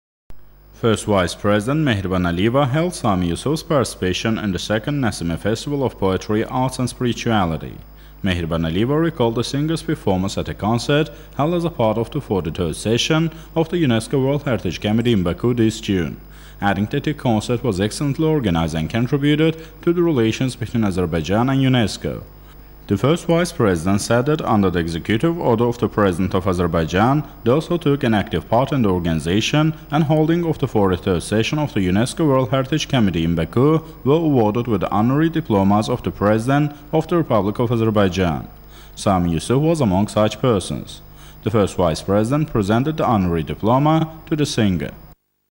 First Vice-President met with world-famous singer and composer Sami Yusuf
First Vice-president of the Republic of Azerbaijan Mehriban Aliyeva met with world-renowned singer and composer of Azerbaijani descent Sami Yusuf, October 1.